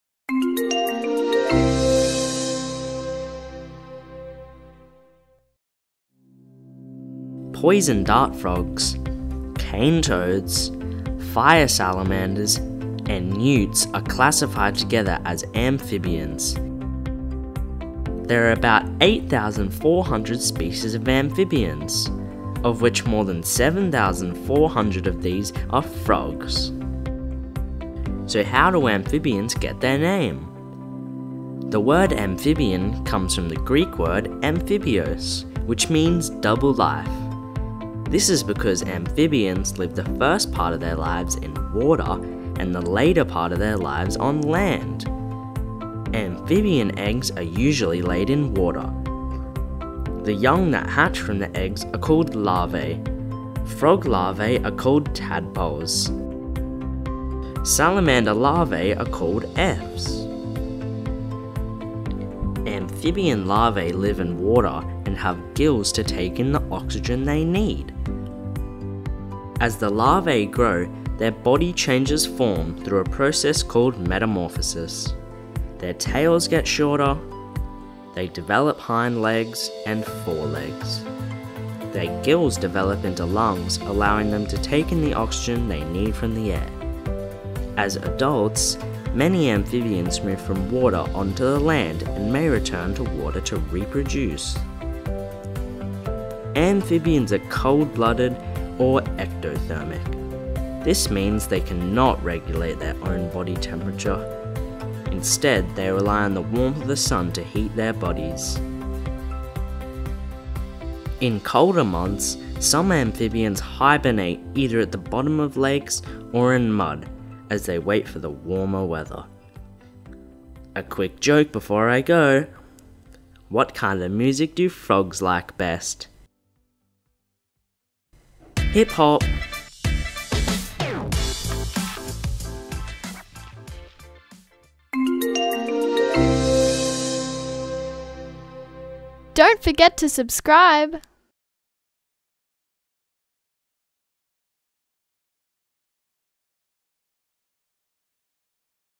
All About Amphibians
Informative video with everything you need to know about amphibians. Head to ngscience.com for full range of related content including worksheets, interactive resources and ebooks.

The word amphibian comes from a Greek term which means ‘double life’. This is due to their unique life cycle which, for many amphibians, begins in water and moves on to land. Amphibians reproduce sexually by external fertilization, and hatch from eggs.

There are three main groups of amphibians. Frogs are amphibians we are most familiar with. They often have short bodies with powerful hind legs for moving about on land and in water. Toads are similar in appearance to frogs, but often have drier and rougher skin.

Salamanders are amphibians that are similar in appearance to lizards. They have long, slender bodies with a tail. Caecilians are limbless, worm-like amphibians. Most live underground.

Most amphibians get the oxygen they need using lungs to breathe in air. Many amphibians are also able to take in oxygen through their moist skin.

The life cycle of a frog is different from other vertebrate animal life cycles in that the young do not look like their parents. As they grow and develop, their bodies change form. This change in body form is called metamorphosis.

Frog eggs are usually laid in water with many eggs usually laid at one time. A frog larva, called a tadpole, hatches from each egg. A tadpole does not look like a frog. It has a tail to help it swim and uses gills to take in oxygen.

As a tadpole grows and develops, it starts to look more like a frog. Its tail gets smaller and it begins to develop legs and feet. Its gills develop into lungs as it prepares to move onto land. As a frog grows, it moves from life in water to life mostly on land.